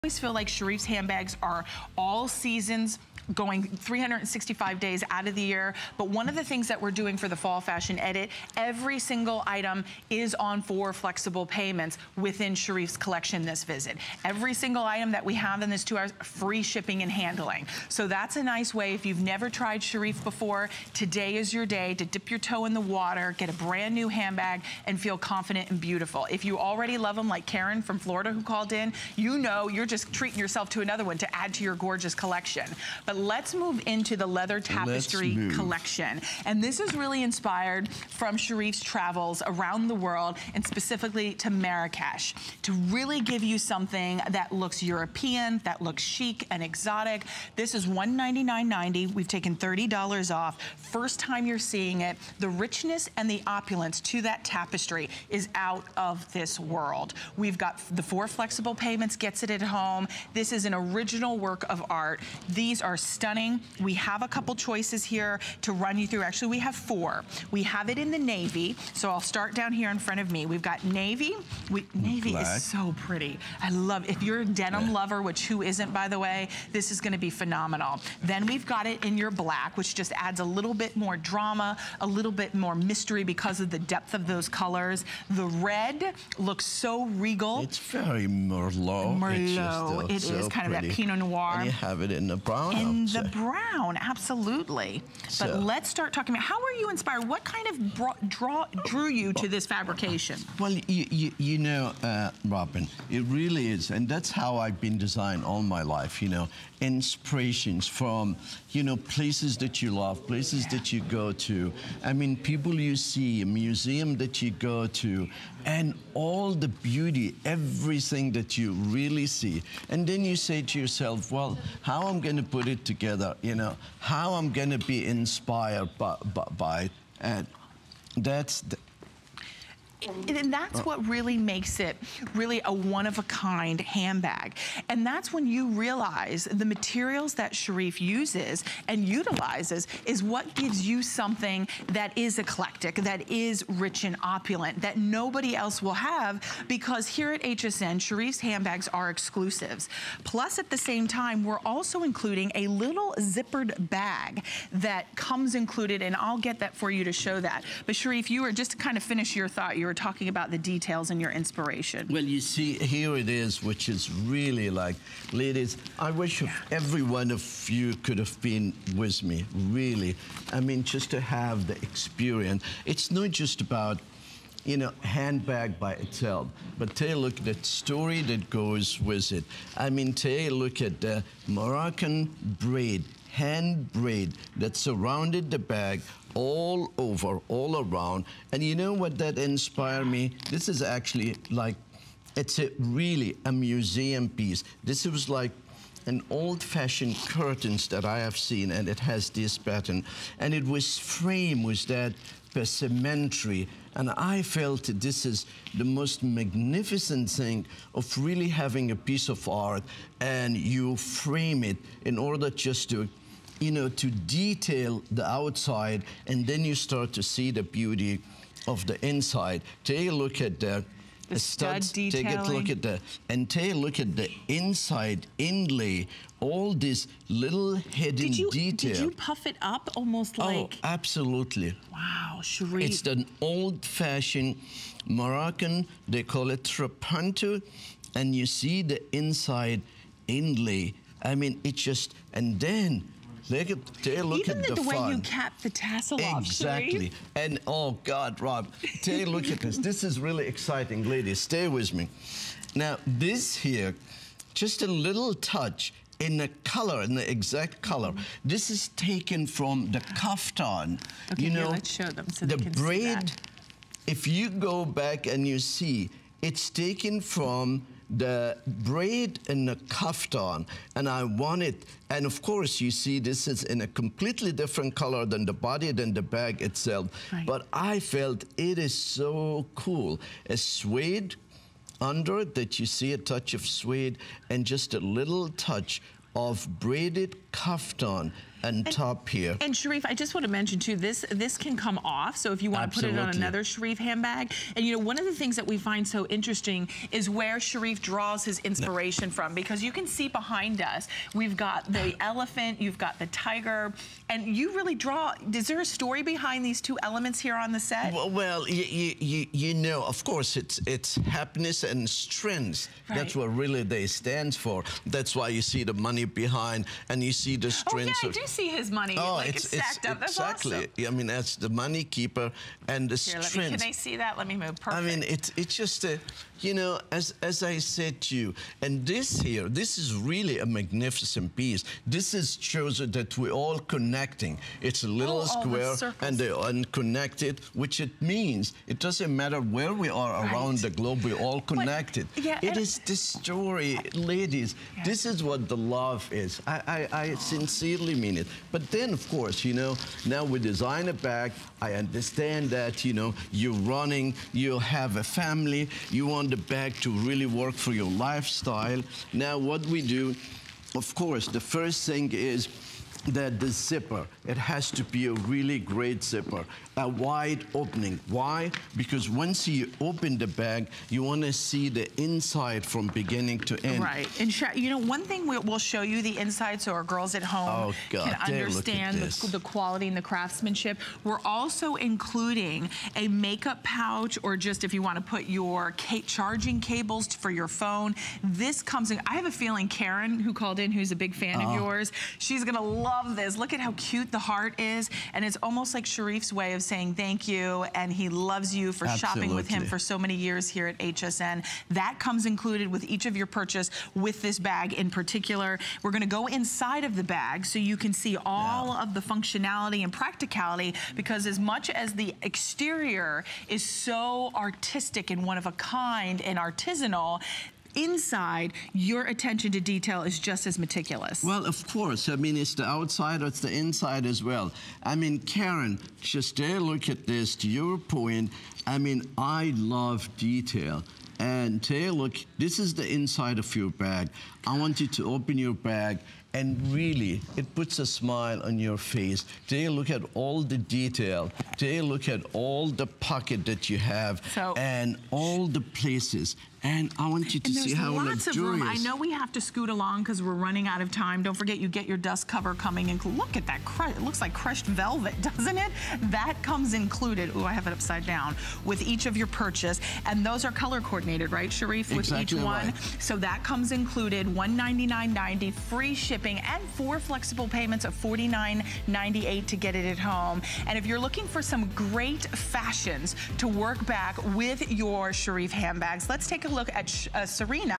Sharif Marrakech Leather Tapestry Satchel Set
Sharif Marrakech Leather Tapestry Satchel with Cosmetic Pouch
From the Love Sharif Collection comes this statement bag crafted from gorgeous tapestry fabric and genuine leather. You'll feel...